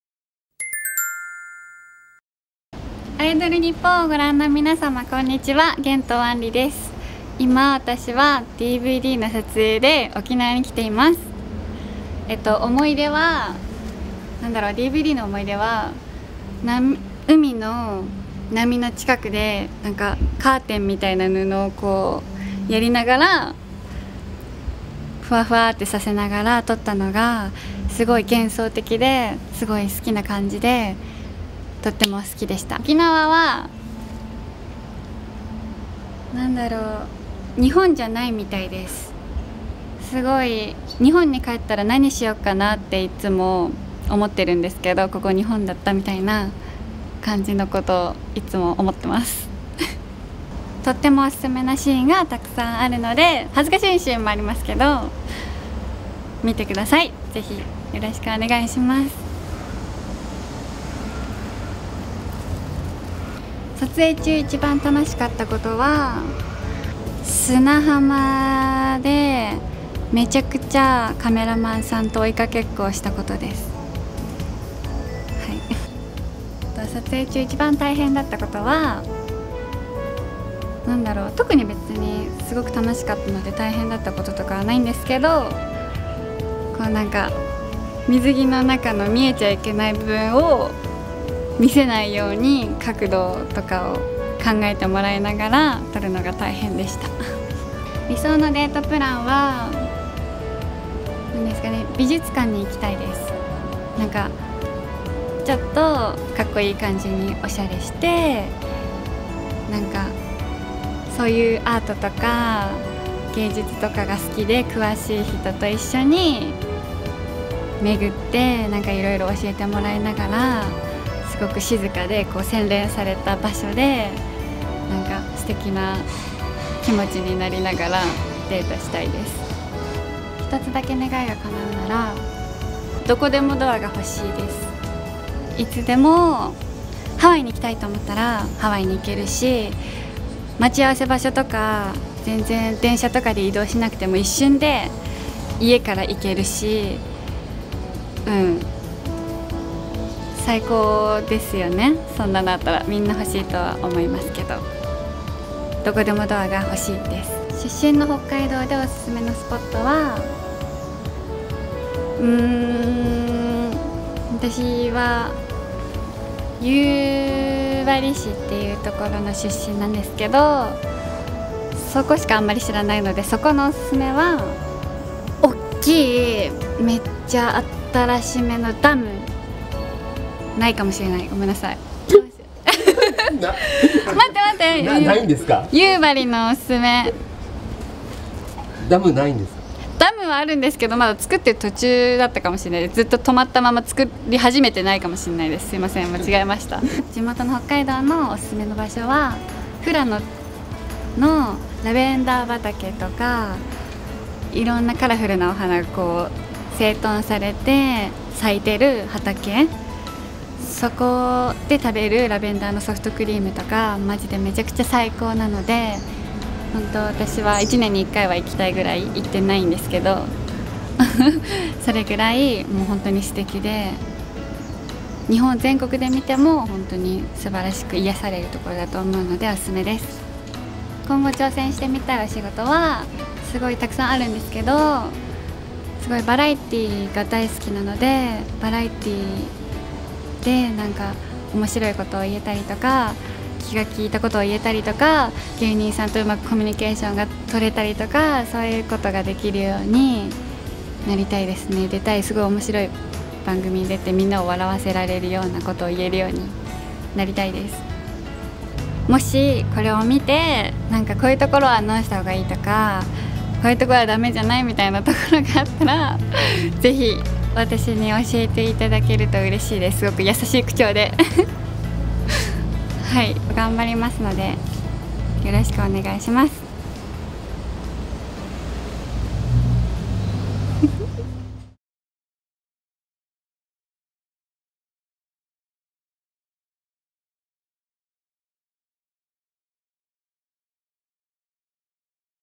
【源藤アンリ】1stDVDコメント＆インタビュー
■BIG COMIC SPIRITS DVD 源藤アンリ「ANRI」
このあざとカワいさは"ウソ"じゃない！！

グラビアやバラエティ番組でマルチに活躍する“あざカワ女子” 源藤アンリ初イメージ作品が登場！！
水着姿のアンリちゃんの透き通る白い肌がまぶしすぎます！
いたずらっぽい笑顔の中に潜む色っぽさに、もうドハマリしちゃうこと間違いなし！！

出演者：源藤アンリ